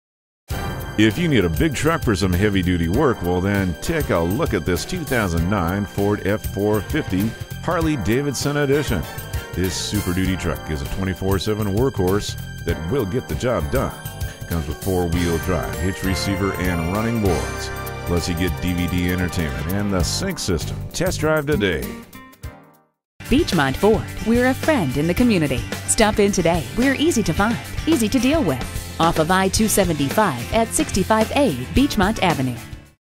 2009 Ford Super Duty F-450 DRW Cincinnati Dayton, OH FT13-2364 - SOLD
Year: 2009
Make: Ford
Model: Super Duty F-450 DRW
Trim: Harley-Davidson
Engine: 8 Cyl. 6.4L
Transmission: Automatic
Color: Dark Blue Pearl Metallic
Interior: Ebony
Mileage: 55848
Stock #: FT13-2364

Address:
600 Ohio Pike